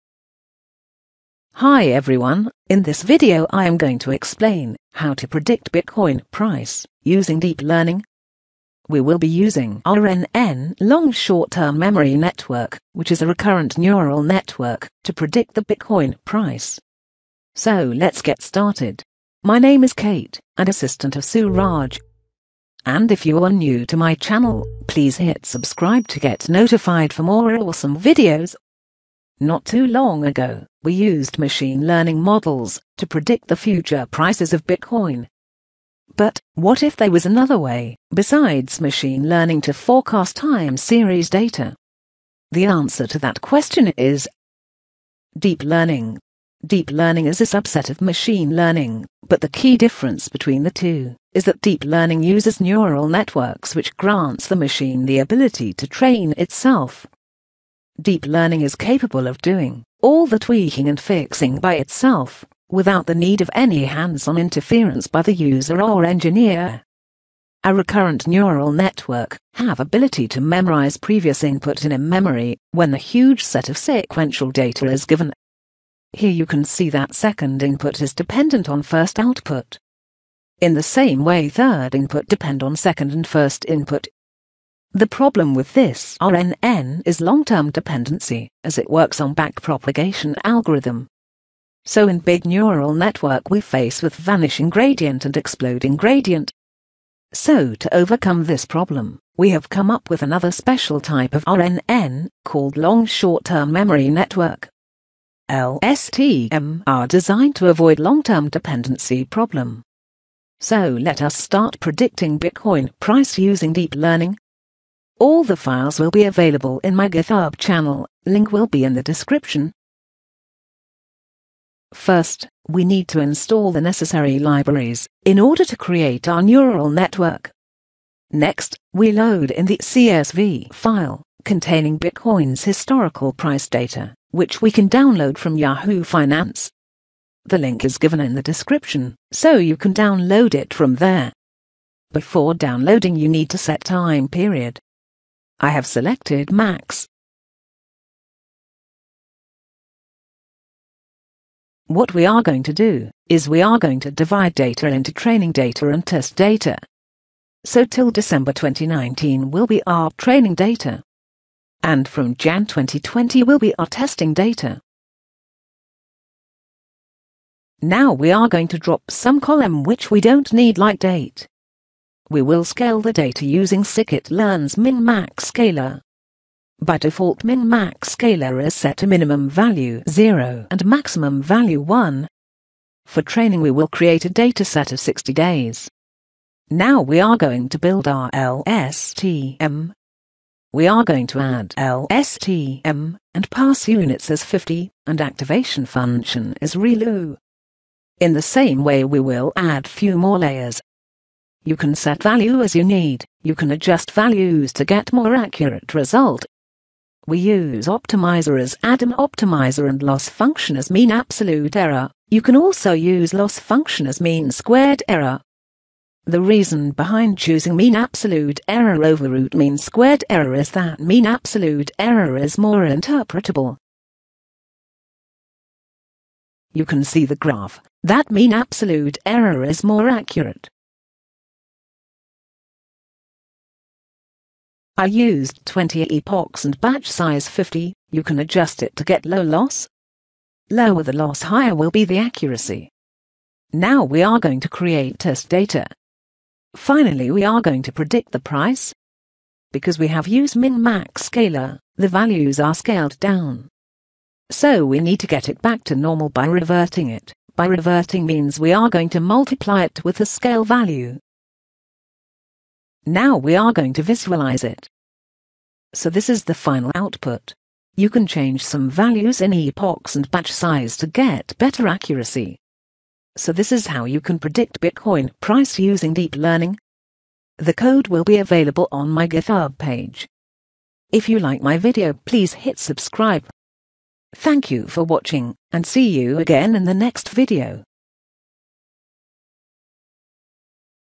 Predicting Bitcoin Price using Deep Learning | Bitcoin Price Prediction using Machine learning
This video deals with Predicting Bitcoin Price with Deep Learning, Using Neural Networks to Forecast Bitcoin Price
Not too long ago, we used Machine Learning models to predict the future prices of Bitcoin.
Although machine learning has been successful in predicting stock market prices through a host of different time series models, its application in predicting cryptocurrency prices has been quite restrictive

In this video, we will be going through a four step process to predict cryptocurrency prices:
1. Getting real-time cryptocurrency data(bitcoin).
2. Prepare data for training and testing.
3. Predict the price of cryptocurrency using LSTM neural network (deep learning).
4. Visualize the prediction results.

we are especially concerned with Bitcoin price Prediction ( Time Series ) using LSTM Recurrent neural network, how we can predict bitcoin price using this model.

you can find the code to CryptoCurrency prediction using Deep Recurrent Neural Networks in my github account.

- suraj bahadur